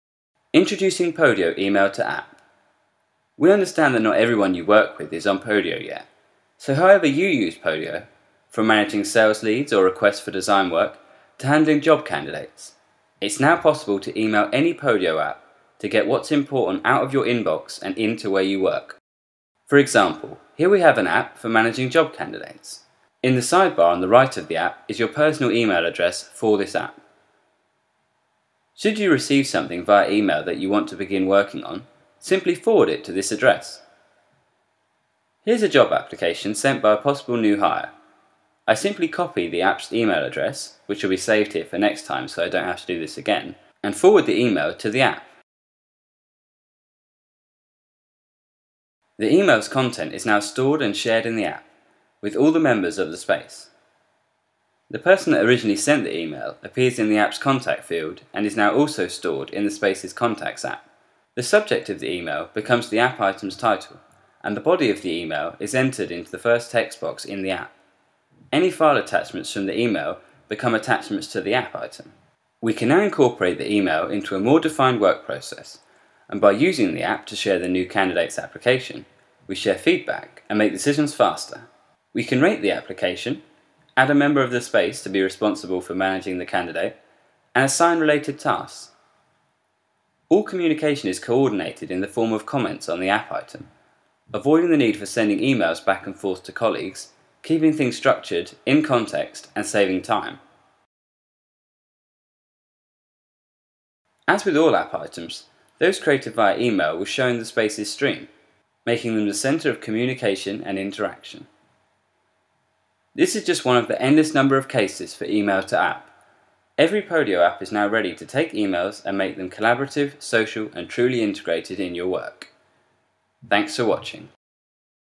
Introducing Podio E-mail to App
It's now possible to e-mail any Podio App to get what's important out of your inbox and into where you work.

Find our more about Podio and sign up for free